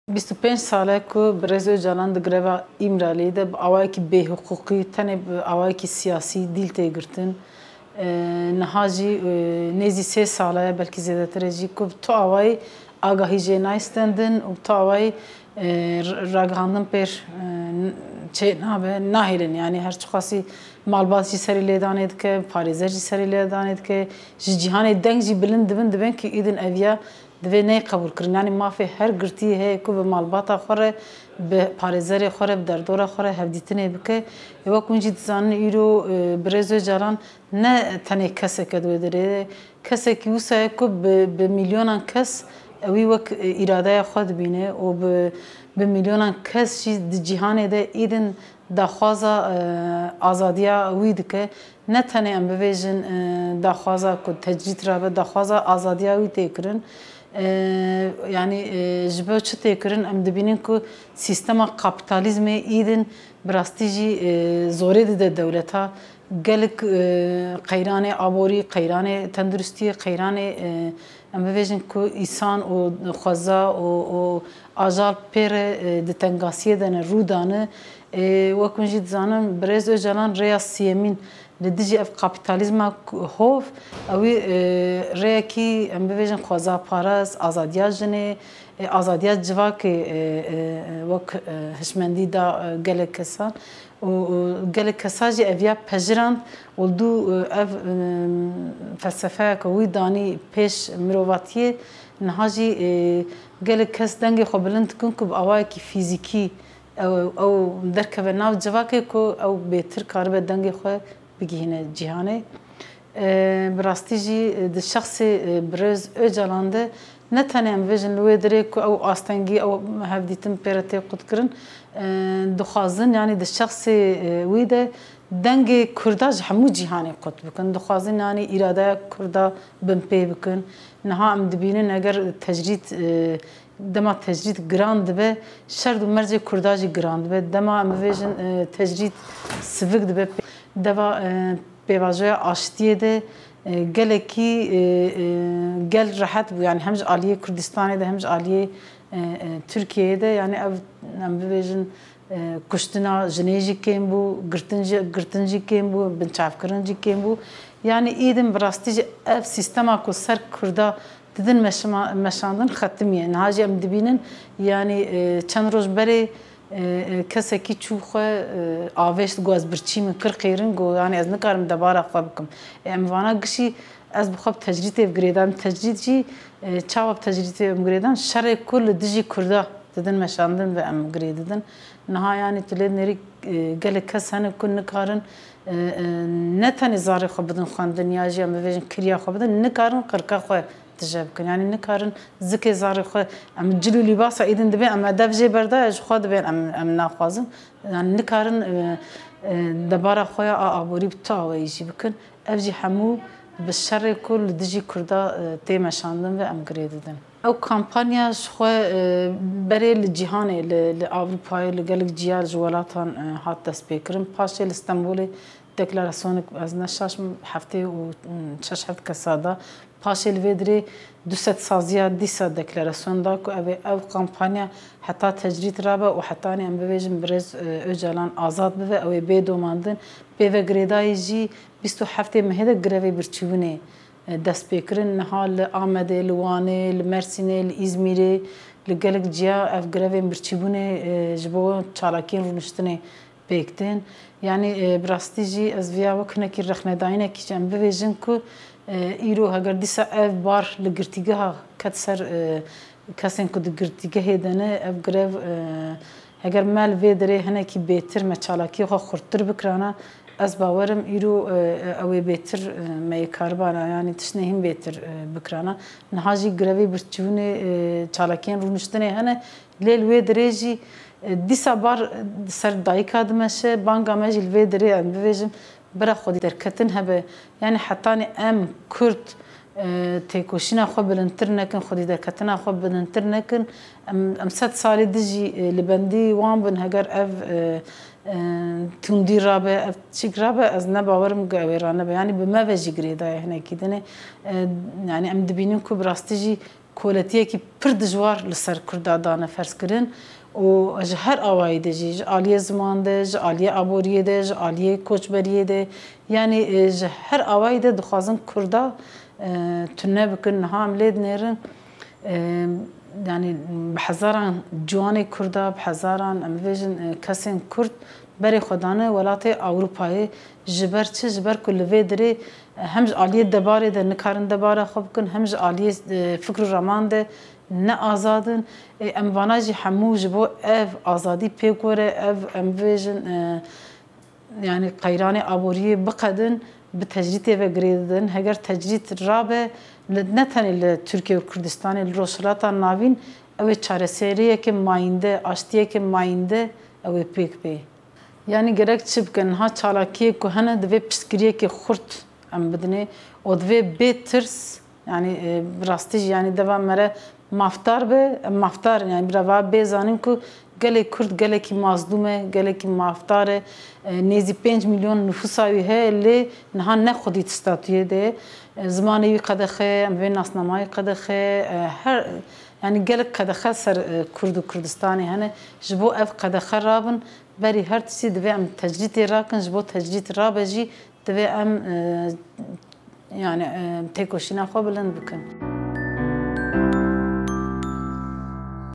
SULTAN YARAY: JI BO AŞTÎ Û ÇARESERIYÊ DIVÊ OCALAN AZAD BE
Hevseroka DBP’a Amedê Sûltan Yaray, diyar kir ku bi milyonan kes ji bo Rêberê PKK’ê Abdullah Ocalan azadiyê dixwazin û destnîşan kir ku ji bo aştî û çareseriyeke mayînde divê tecrîd bê rakirin.